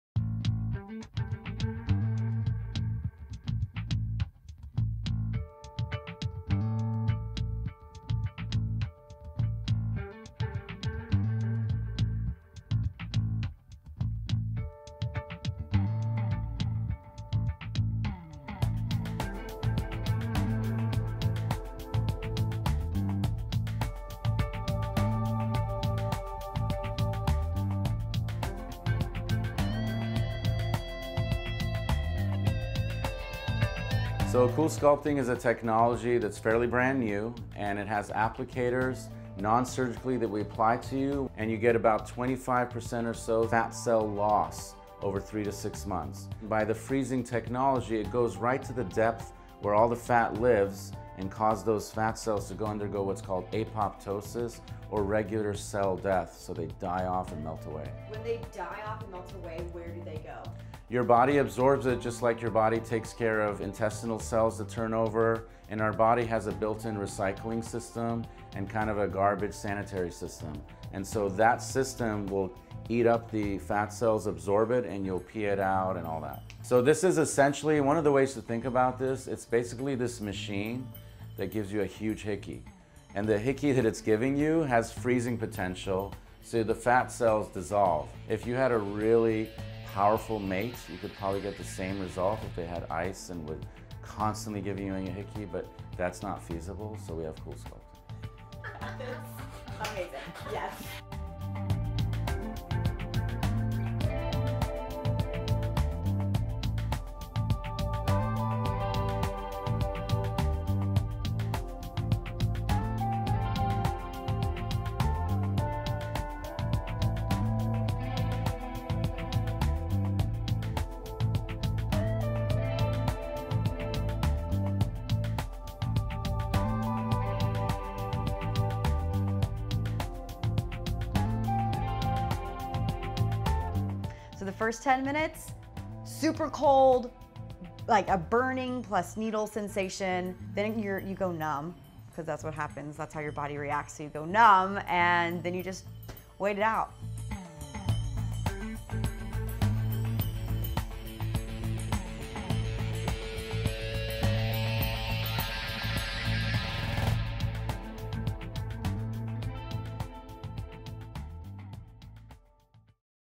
My Coolsculpting (Fat Freeze) Experience - Is it worth the money?
Freeze away your fat! Eliminate stubborn fat without surgery or downtime. The procedure is FDA-cleared, safe and effective.